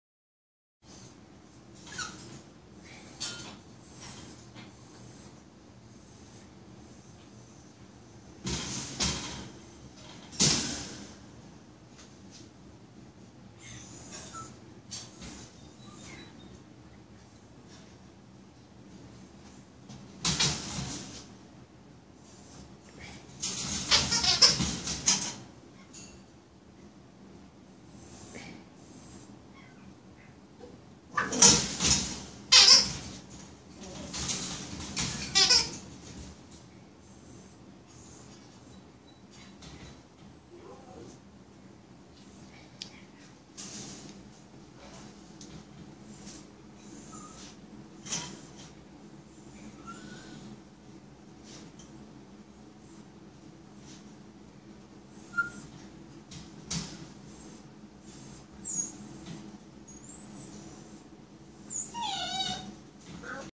Ear crop puppy
A few hours post ear cropping, a happy, playfull puppy!